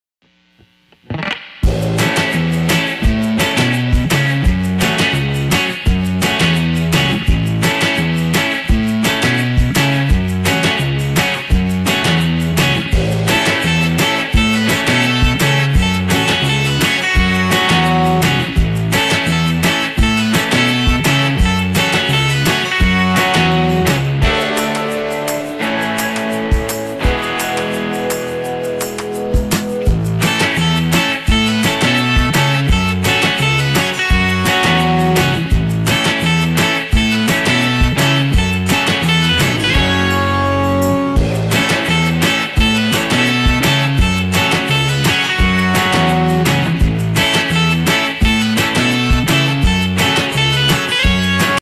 Moon Stone 1: It Came From the Swamp (black and white version)
Frank Rock ventures deep into the monster realm looking for the Swamp Creature Moonstone! Black and white old timey filter is on, so it's hard to tell but the baseplate is blue for water!